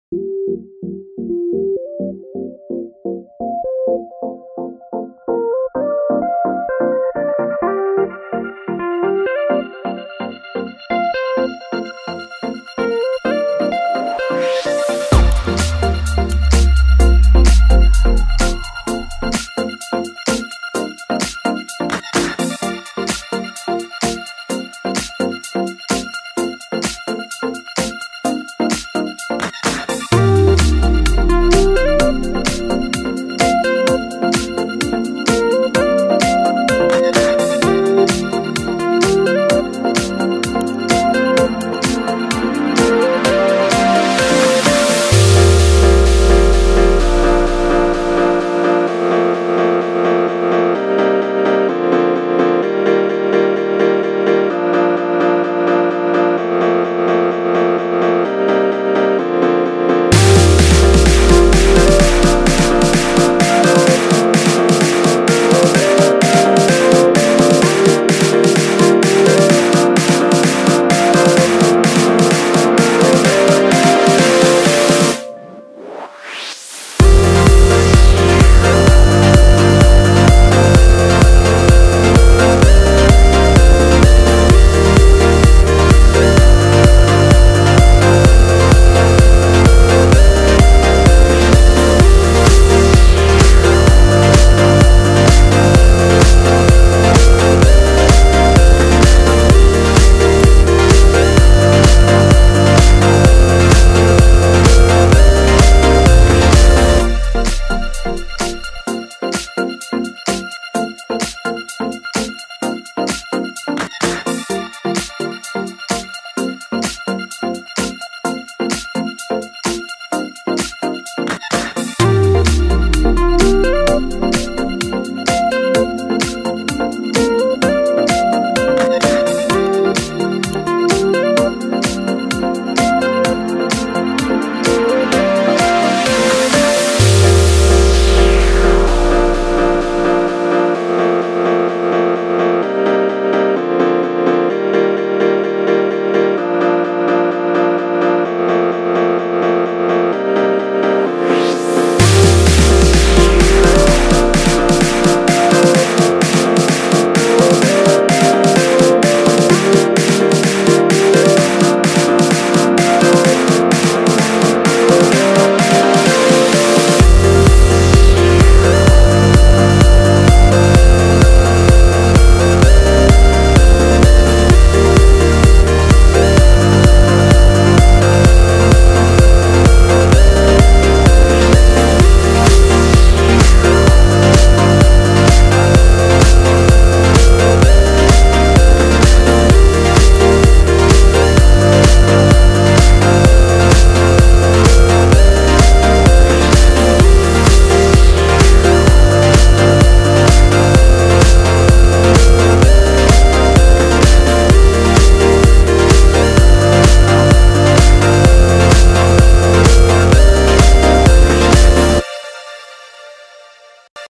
Dibujando a Mei Lee de "Red" paso a paso
En este video, muestro cómo dibujo a Mei Lee, la protagonista de la película animada "Red". Desde el boceto inicial hasta el resultado final, podrás seguir todo el proceso y aprender algunos consejos útiles para mejorar tus propias habilidades de dibujo. ¡No te lo pierdas!